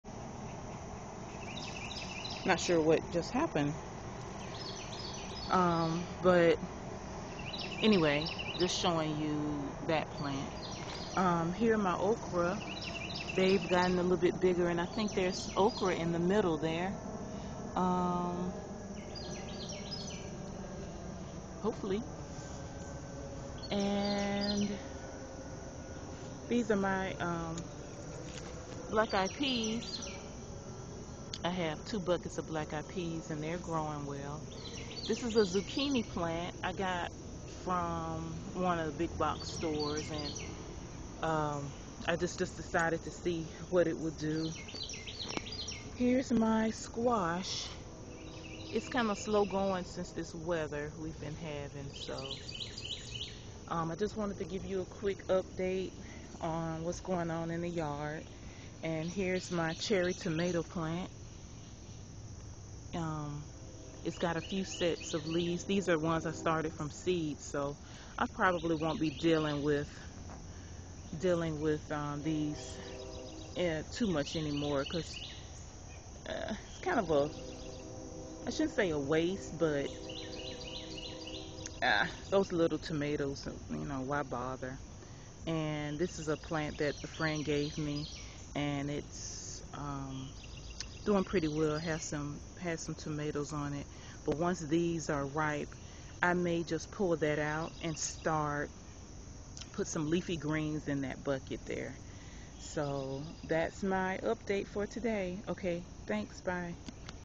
Continuation of Container Garden update 7/18/12
We have gone from being in high temps of 100s to rain storms everyday in the past week. Can't fool with nature.